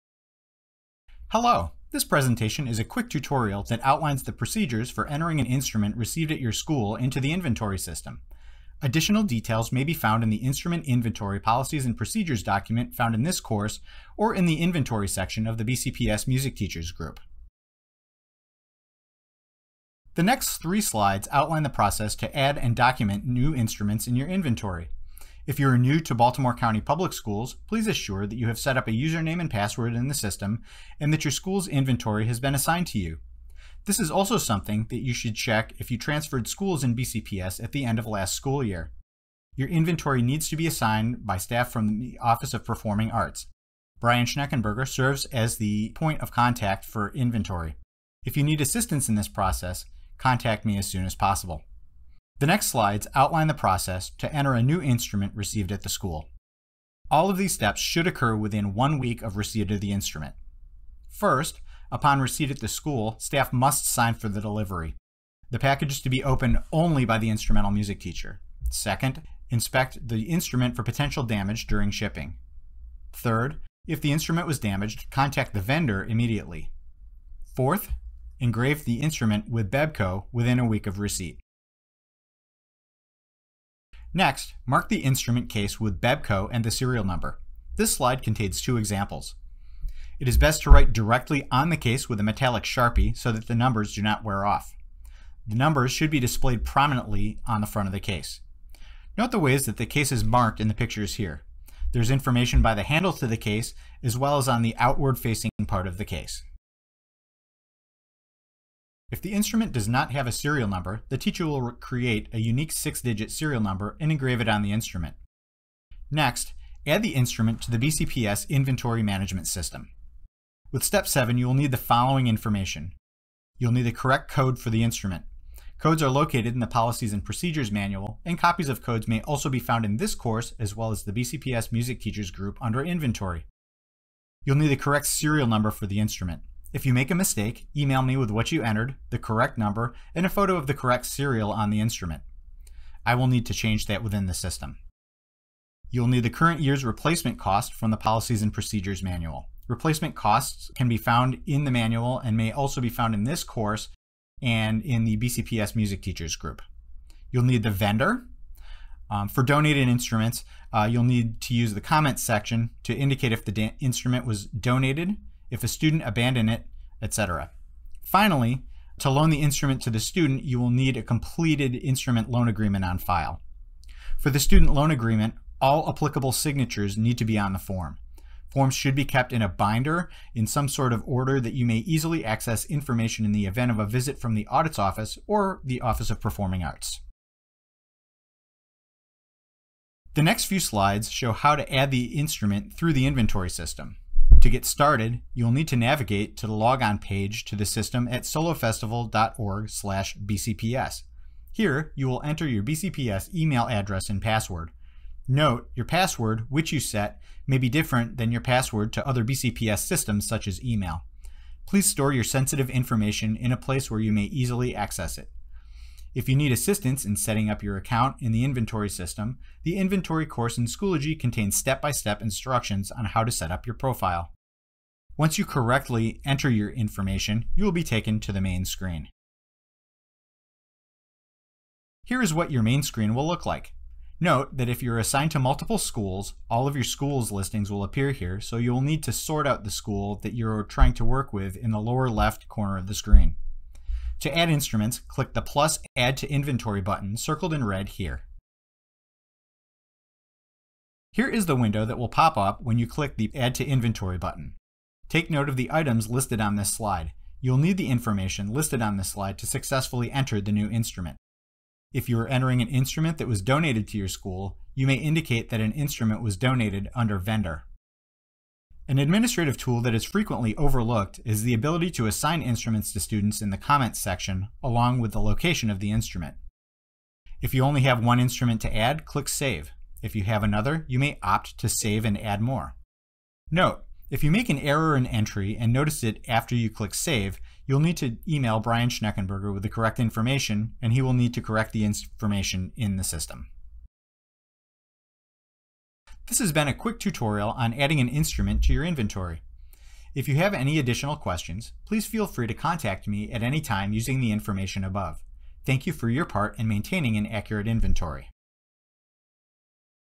Receipt of New Instrument 2022-2023
This video tutorial outlines the process to enter a new musical instrument into the inventory system for the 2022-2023 school year.